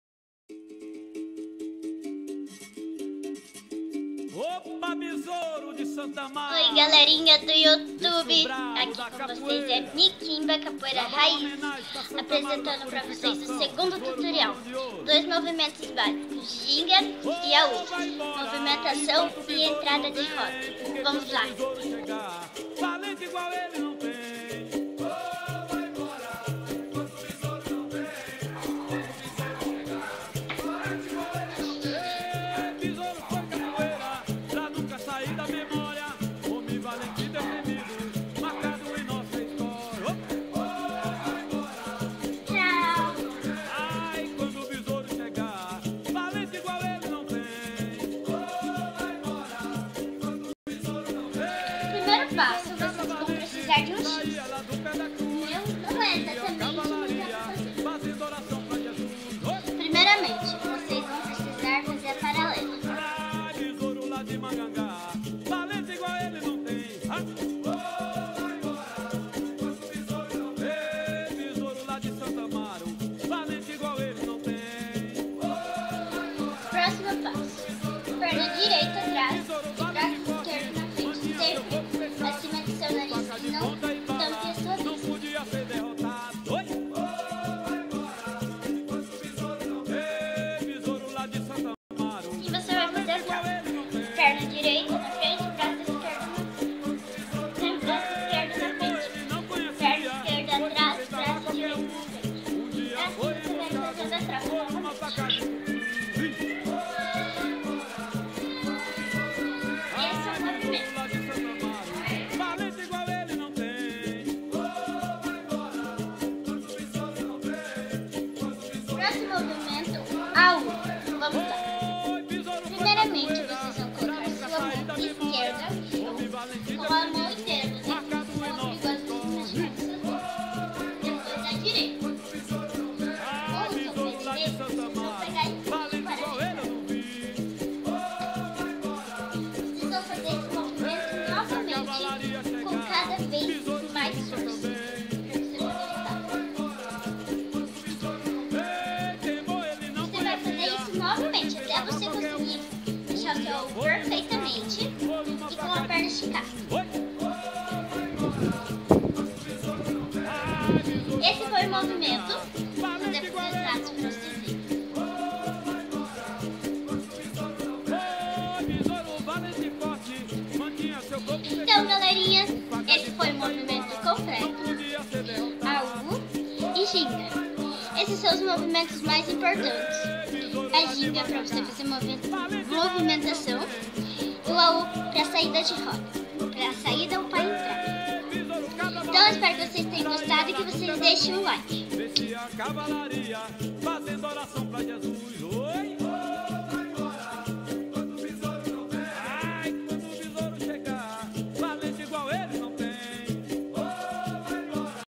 Tutorial Ginga e Aú com Mikinba Gp Raízes da Tradição.
Tutorial entrada e movimentação básica.